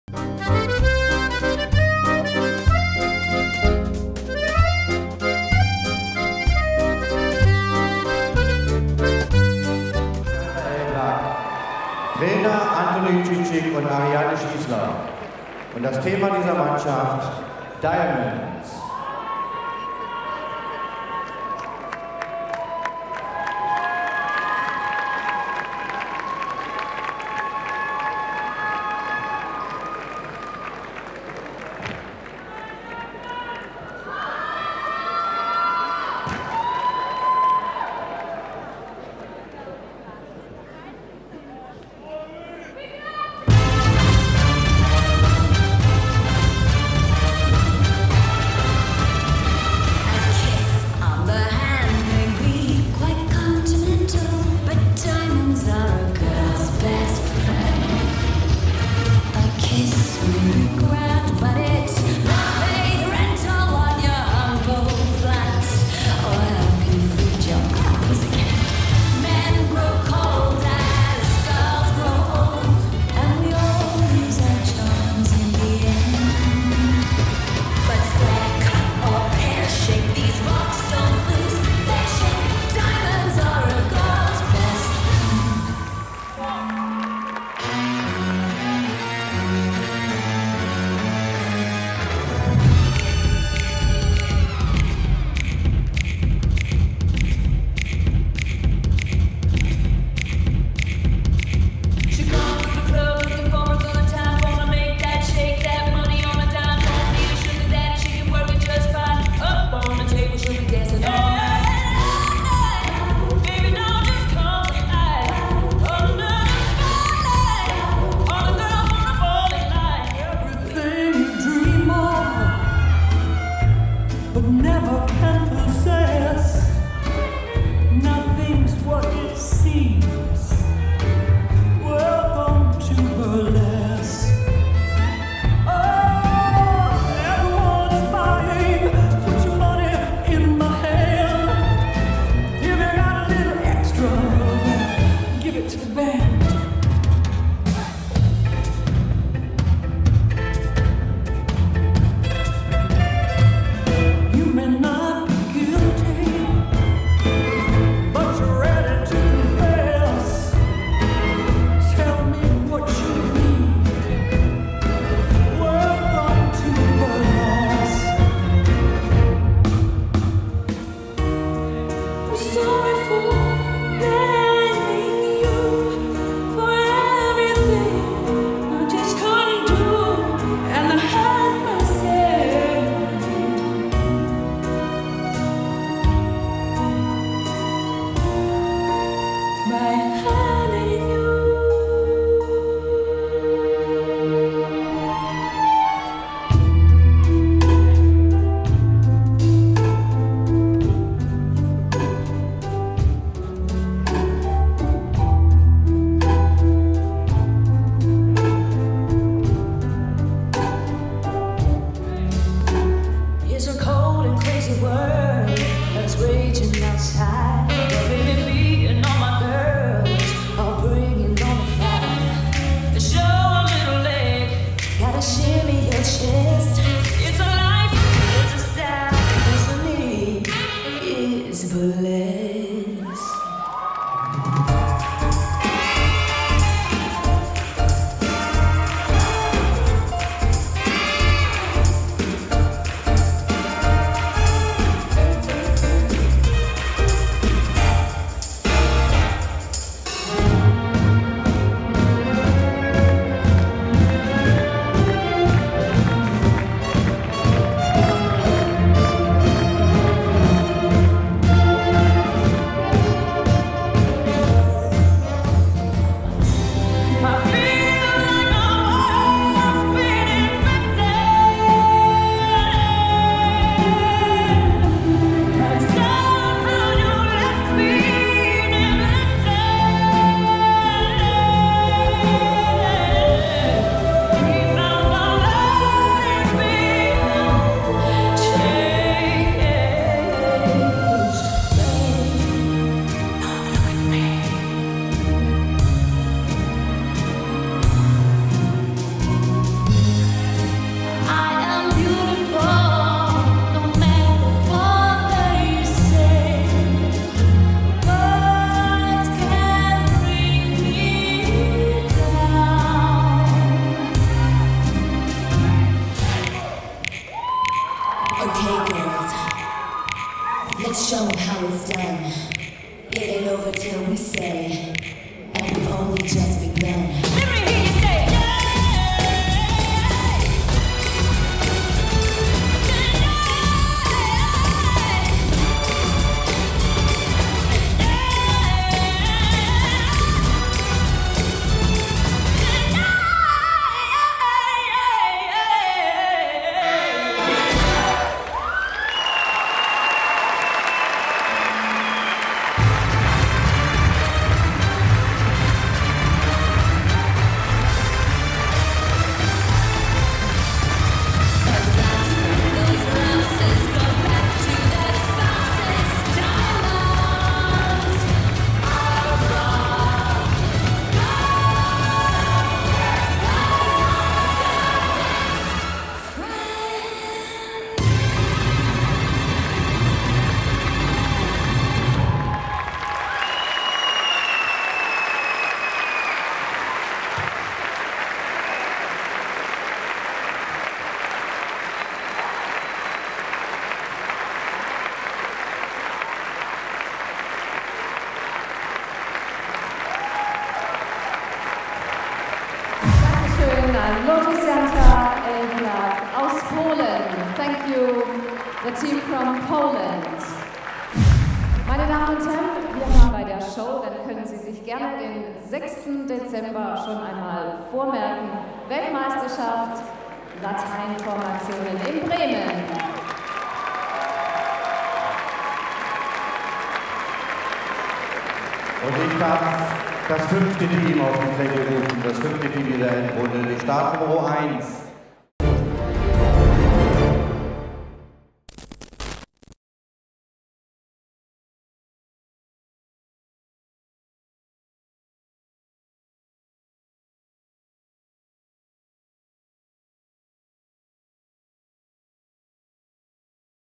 LOTOS-Jantar, POL | 2014 World Formation Standard | DanceSport Total
LOTOS-Jantar, POL | 2014 World Formation Standard

LOTOS-Jantar, POL, dance the final at the 2014 WDSF World Formation Standard in Braunschweig, GER on 1 November.